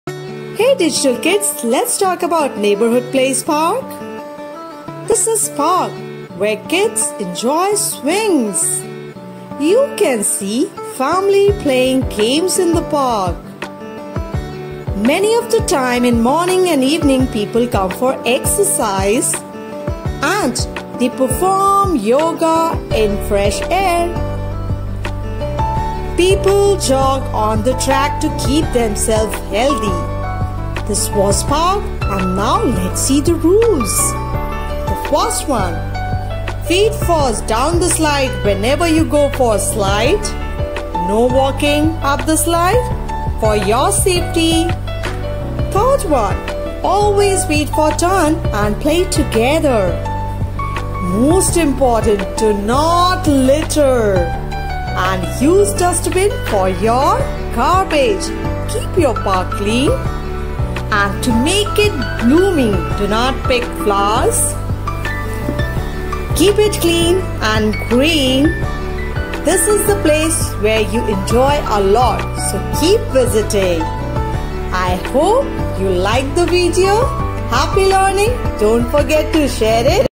Neighbour Place PARK | Teach your kids the places around | neighbourhood | preschool | Digital MOM
Digital MOM # Pre Primary
A park is a public area of land with grass and trees, usually in a town, where people go in order to relax and enjoy themselves. ... When you park a vehicle or park somewhere, you drive the vehicle into a position where it can stay for a period of time, and leave it there.

STAY WITH US TO THE END!

Musician: @iksonmusic